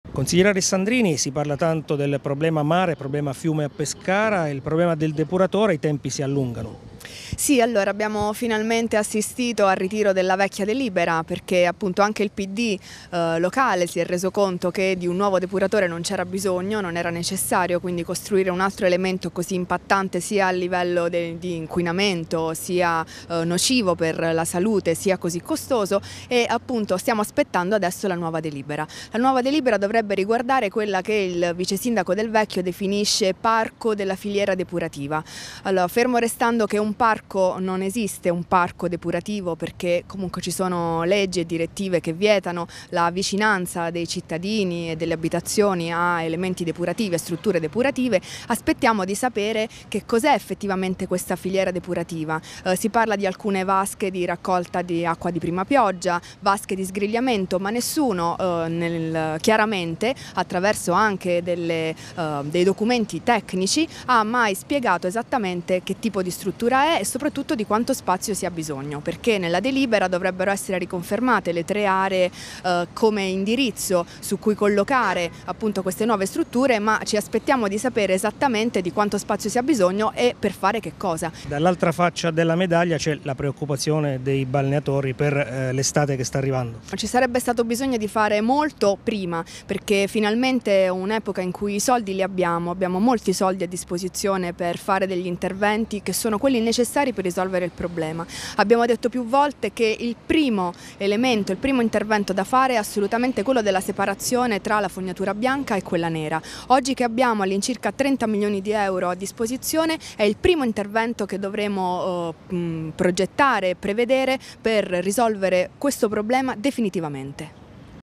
M5S: grave i ritardi sul depuratore
La consigliera comunale del  M5S Erika Alessandrini giudica preoccupante la situazione riguardante il depuratore di Pescara e i nuovi lavori di adeguamento